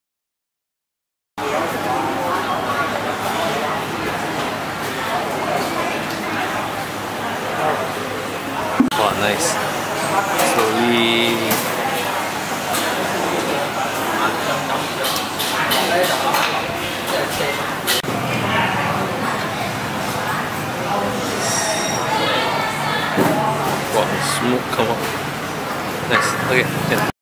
Where to Makan - Pork Noodles at Kim Kee Restaurant, Kuantan (金记豬肉粉)
金记豬肉粉  is one of the best pork  noodles in Kuantan. Usually, you will have to take a number when you order it. RM5.50 per bowl, very generous serving. No fish balls in it. I hate it when they put fish balls or fish cake inside Pork Noodles.

GEARLIST
Main Camera :  Lumix G95
Secondary Camera: Samsung NX300m
Tripod : Joby Gorillapod
Main Phone : Honour 8x (PokemonGo)
Secondary Phone : HuaWei Nova 2 Lite (PokemonGo)
Drone: DJI Spark